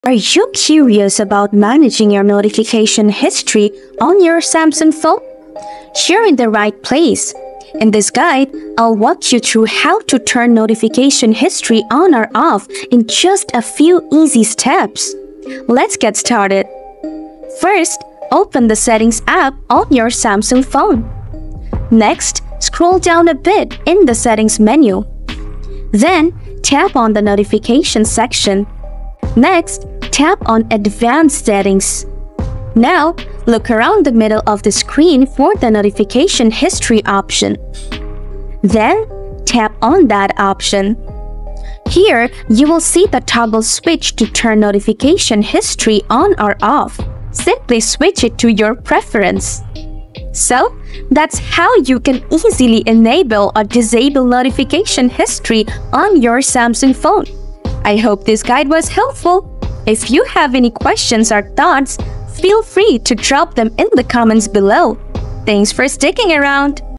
How to Enable or Disable Notification History on Samsung Phone | Turn On/Off Notification History
Need to Enable or Disable Notification History on your Samsung Phone? No worries, watch me! If you wanna Activate notification history on Samsung, simply watch the video guide. In this advanced video guide, I'm gonna show you how to turn off notification history on Samsung and the right method to enable notification history on Samsung as well.

Watch the entire video and follow the instructions step by step carefully to get rid of Samsung notification history.

00:01- Video intro
00:17- The Solution
00:50- Solution ending